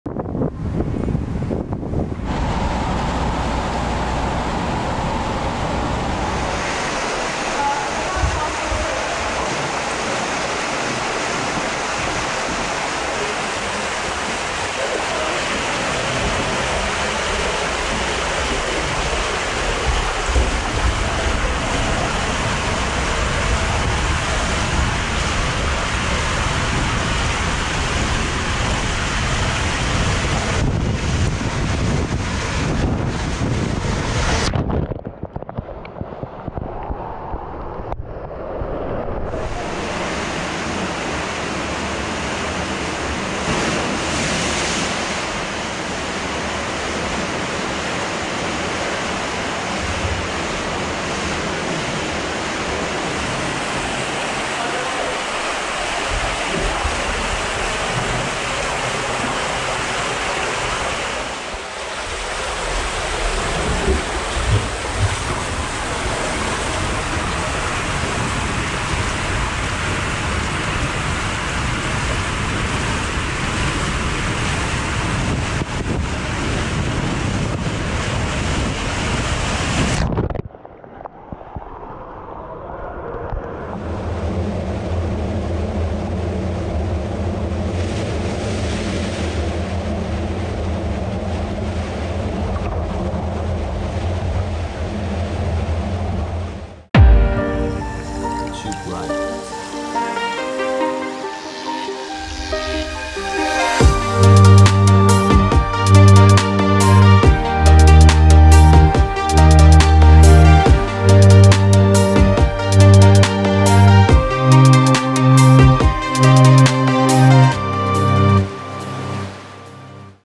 Fast AQUATUBE Water Slide at Kecskeméti Fürdő
This is the Aquatube water slide at Kecskeméti Fürdő in Kecskemét, Hungary. The slide is fitted with colorful daylight effects and is situated in the facility's indoor water park.

Type: Aquatube
↔ 86.47 m
↕ 12 m
Manufactured by: Polin (distributed by Eleven Kft.)